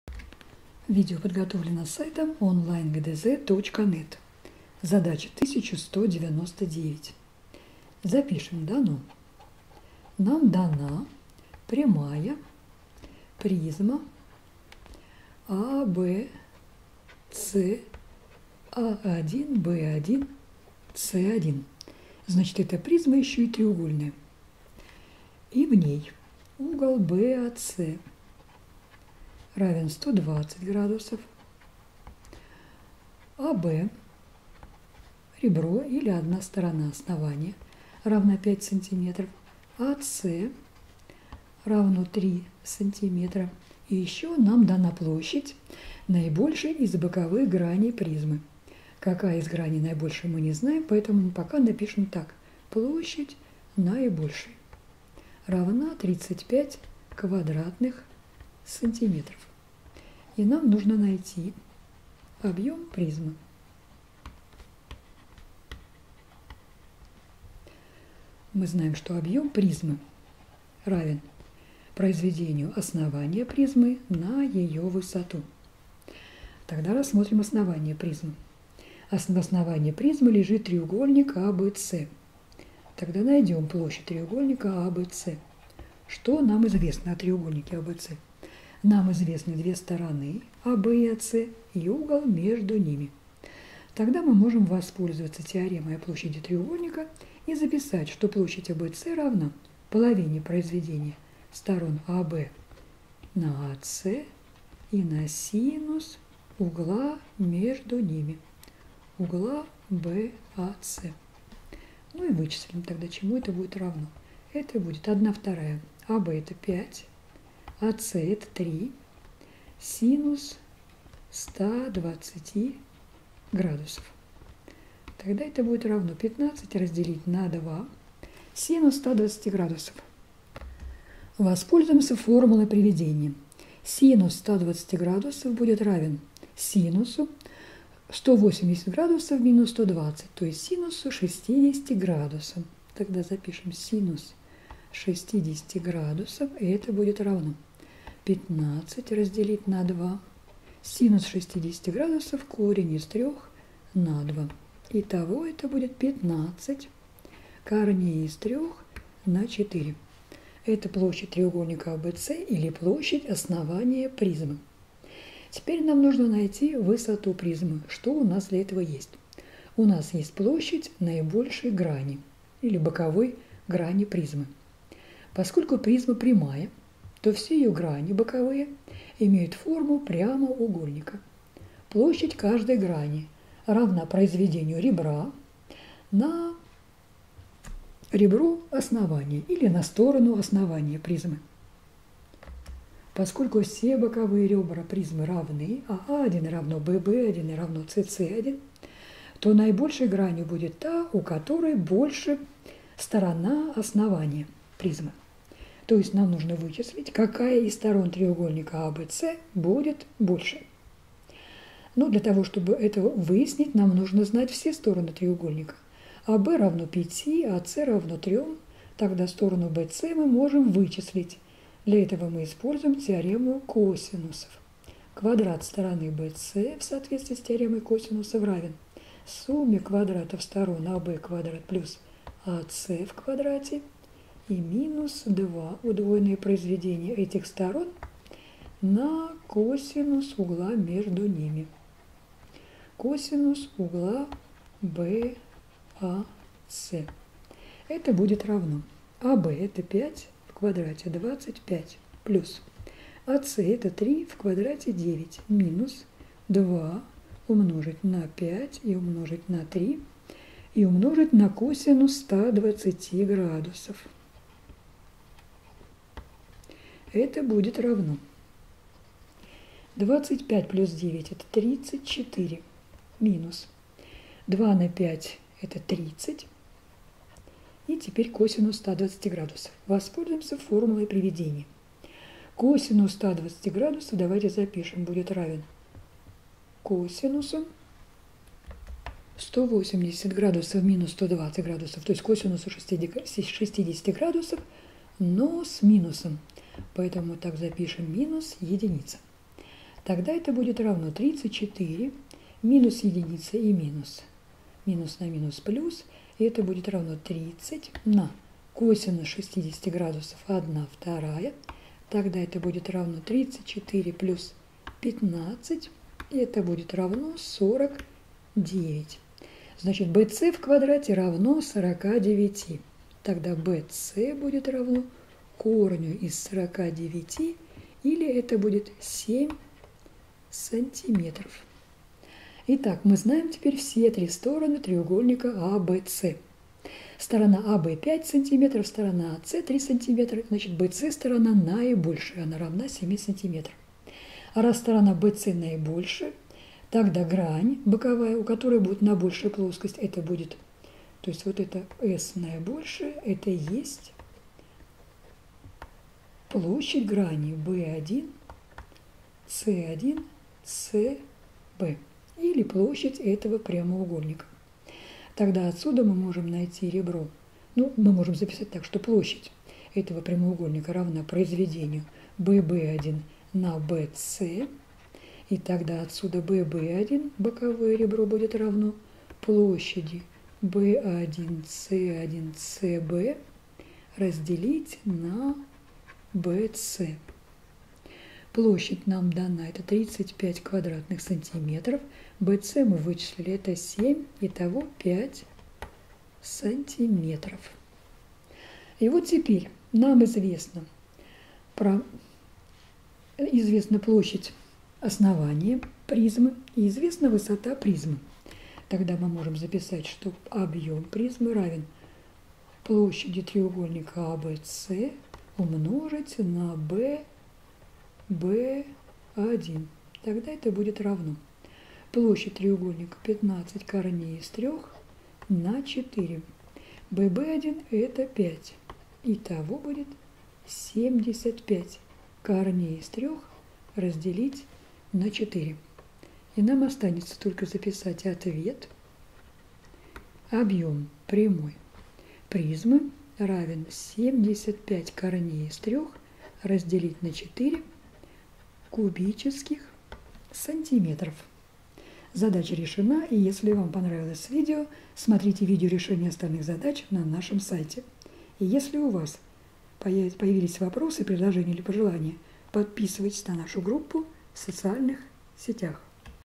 Задание № 1199 - Геометрия 9 класс (Атанасян)
Для решения задачи 1199 нужно воспользоваться формулой для объема призмы: объем призмы равен произведению площади основания на высоту ( см. п. 123 из 1 параграфа 14 главы). Чтобы сделать это задание без ошибок, воспользуйтесь нашим видео с решением.